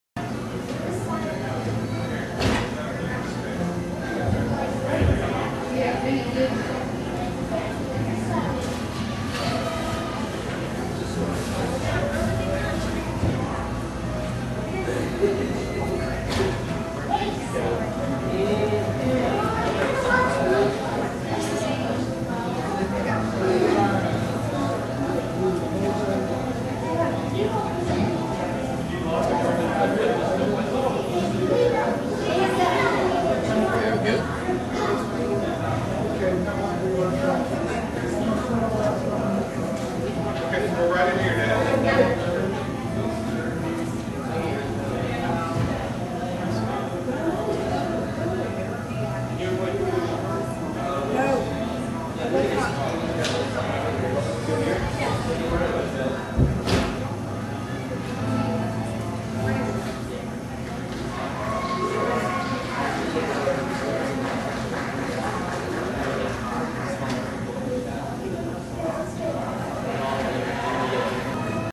Judges Cup, Beam, 10/21/07
Beam score 9.050 and placed 3rd